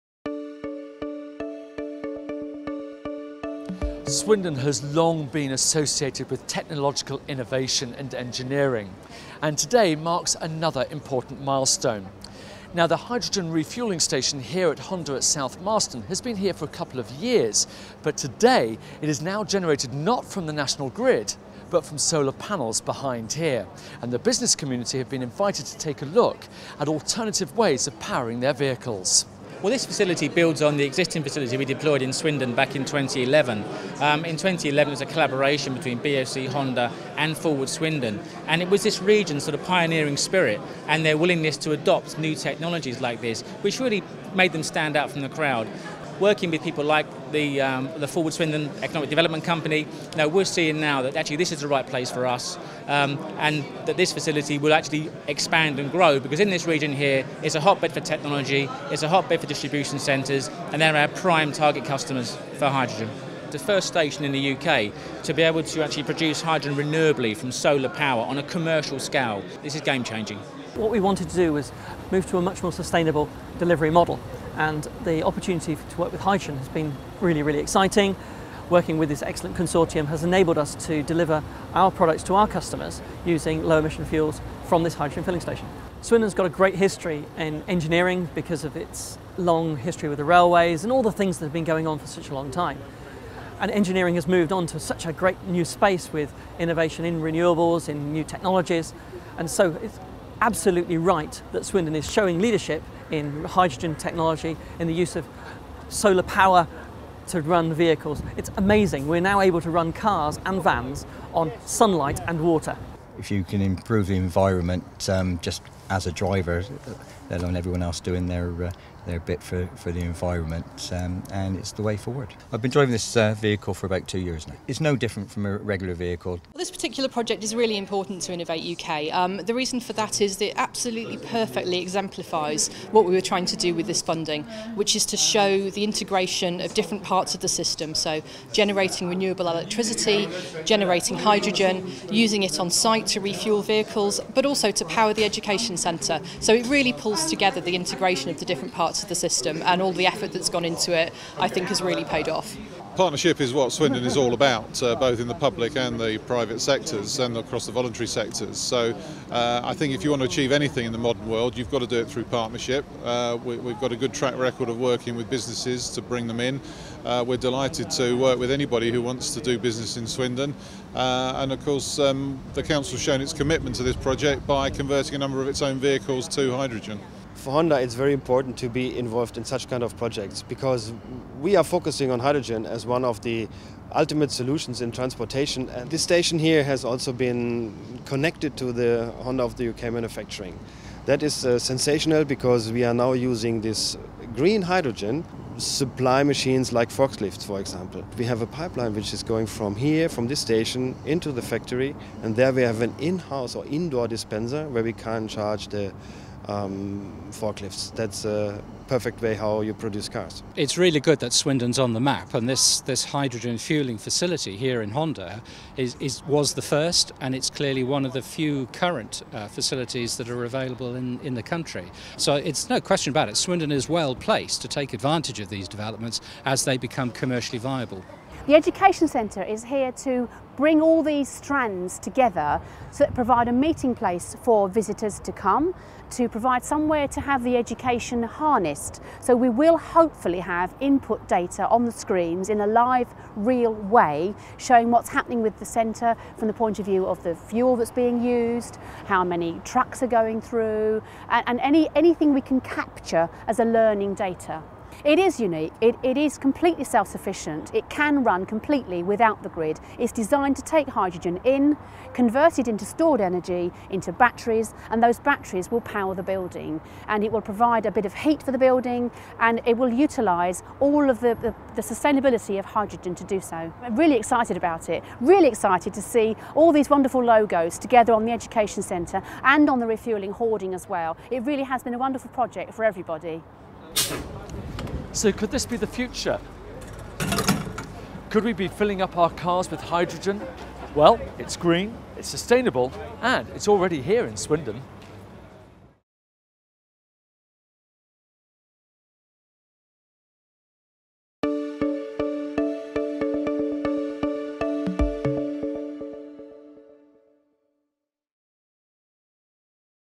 Hydrogen Fuel is produced from Solar Energy in Swindon, England UK
First green hydrogen refuelling station opens

Solar-powered hydrogen available to drivers at Honda's Swindon factory

Gas supplier BOC has opened the UK's first “green” commercial-scale hydrogen refuelling station at the Honda factory in Swindon to test the commercial viability of the technology.

 The refuelling station at Swindon uses electricity from a nearby 15MW capacity solar photovoltaic plant to power an industrial electrolyser, which produces hydrogen from water to be dispensed for several applications.

 The hydrogen leaves the electrolyser, which was manufactured by Belgian company Hydrogenics and consumes around 4,9 kWh/Nm3 of electricity, at 9bar. The hydrogen is then compressed in a single stage up to 900bar and stored for use in several applications: a dual pressure refuelling pump for cars that use hydrogen at 700bar and commercial vehicles at 350bar; a forklift refuelling station within the honda factory operating at 350bar, and a small building which uses a M-Field fuel cell system operating at around 5bar to supply heat and light.

 The hydrogen station can produce and dispense 3kg of hydrogen an hour, up to 200kg a day, and store up to 135kg. When the pressure drops in the hydrogen store, the system responds by producing more hydrogen to repressurise and “top-up” the storage. The hydrogen production and storage process takes around 90 minutes from start to finish...

Solar Hydrogen: Fuel of the Future

Video re uploaded from Swindon to help spread the word. Copyright is owned by Swindon.